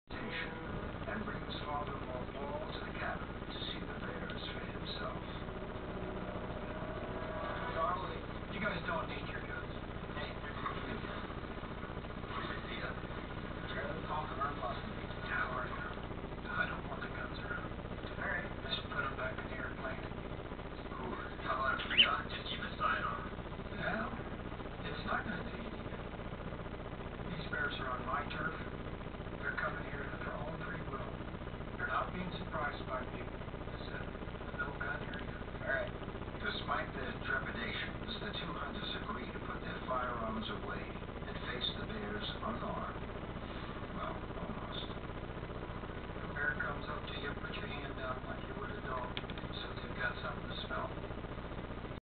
Cockatiels Elvis and Fabian play with a toothpick
It's one of their favorite toys, but I always cut the pointy ends off before giving one to them.